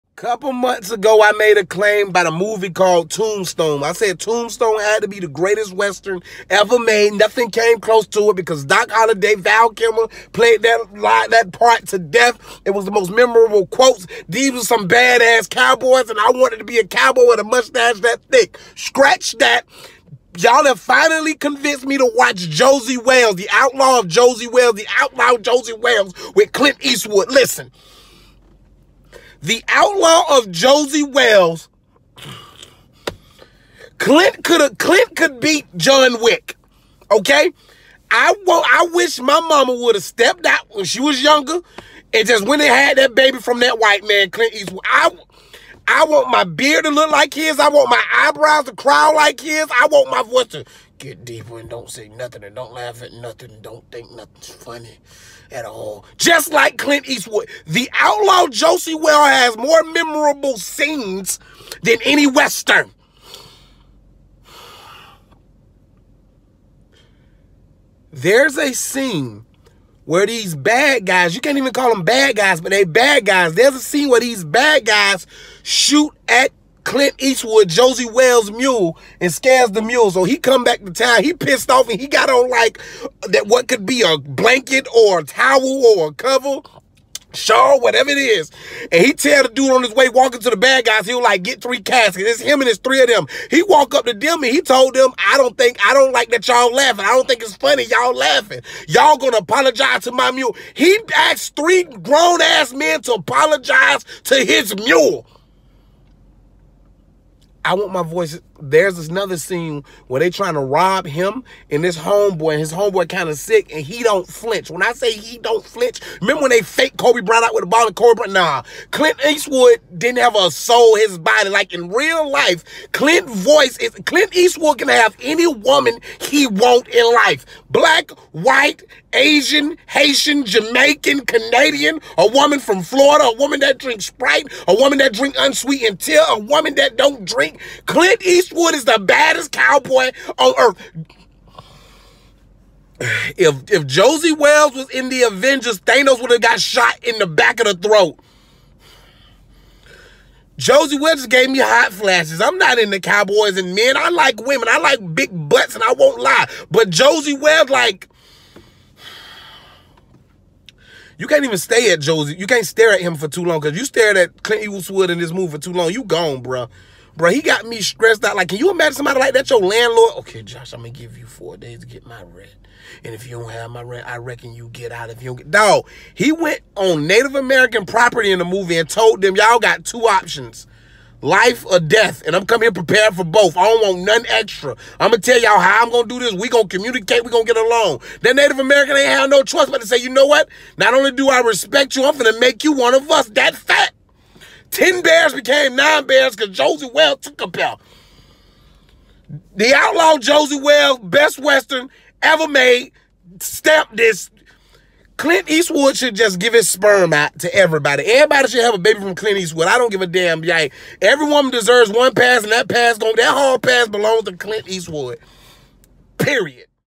The Outlaw Josey Wales
Comedian Josh Pray thinks Clint Eastwood is really Josey Wales in real life